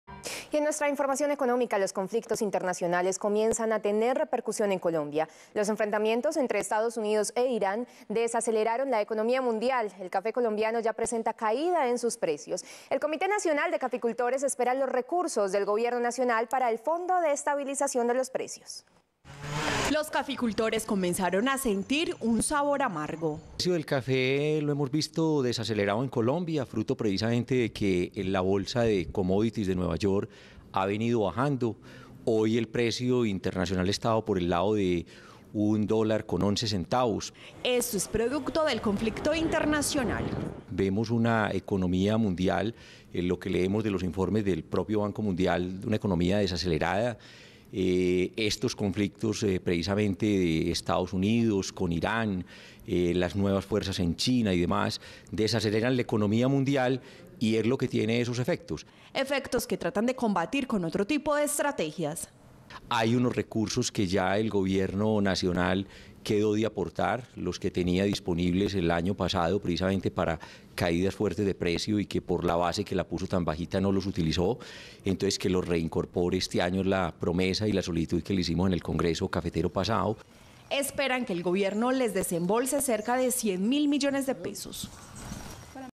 El conflicto entre Estados Unidos e Irán llega a Colombia
Los conflictos internacionales comienzan a tener repercusión en Colombia.
Los enfrentamientos entre Estados Unidos e Irán desaceleraron la economía mundial, el café colombiano ya presenta caída en sus precios.